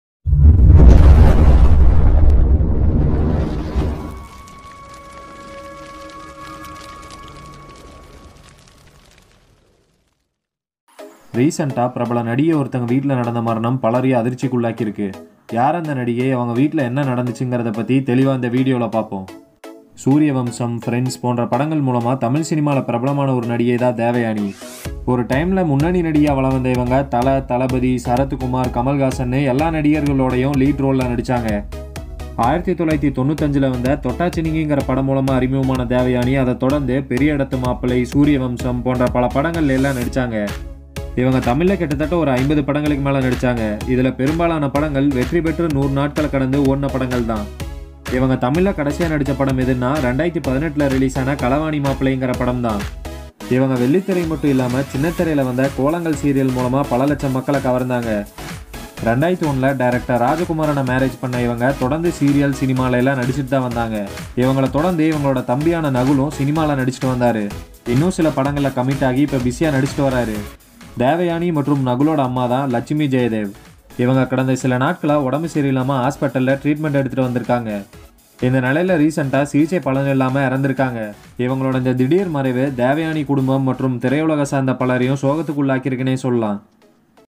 sad incident happened to devayani recently
தேவயாணி வீட்டில் நடந்த துக்கம்!

Devayani is a leading actress in 90's period of time. In this video we are going to see about the sad incident happened to devayani recently.